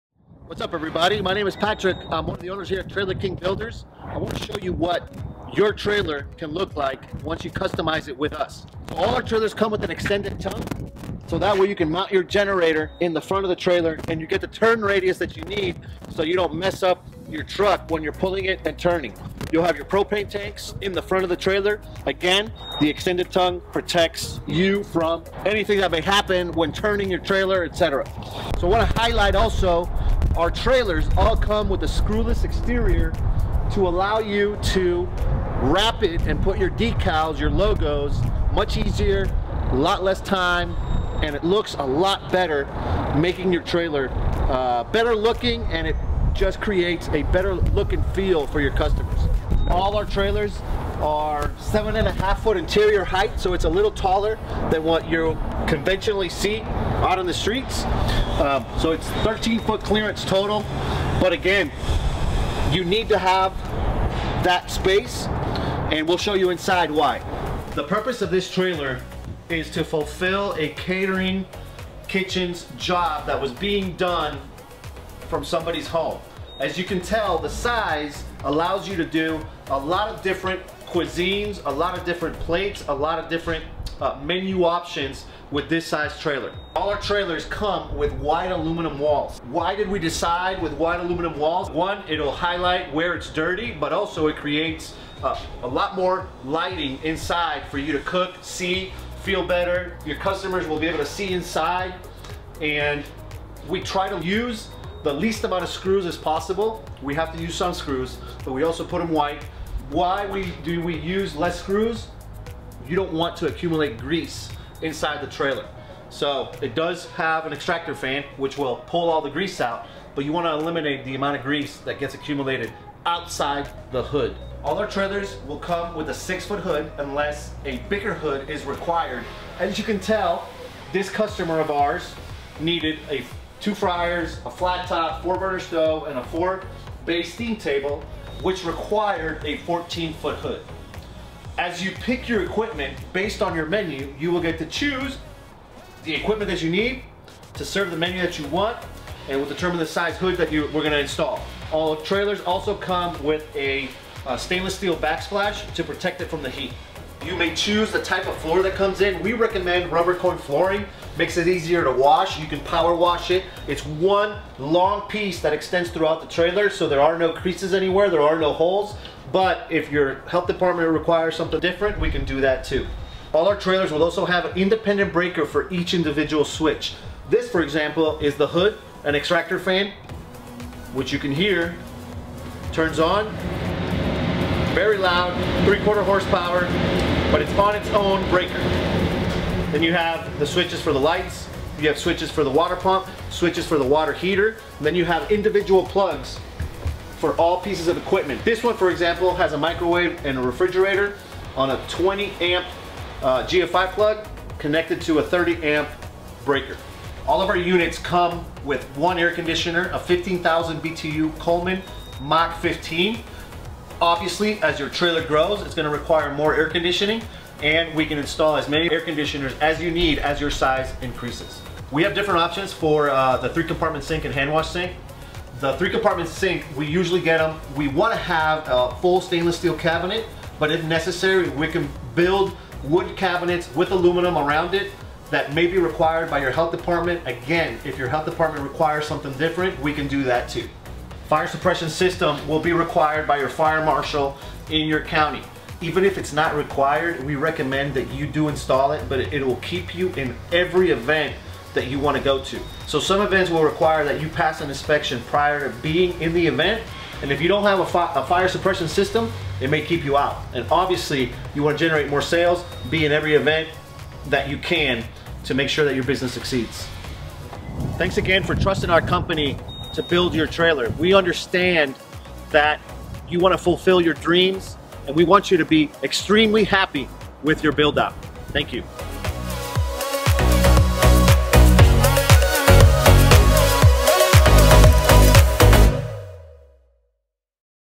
What's In Our Trailer?
this is what you can expect to get when purchasing a trailer from us.

the attention to details is what we take pride in.

you will will be happy with the trailer you get from Trailer King Builders.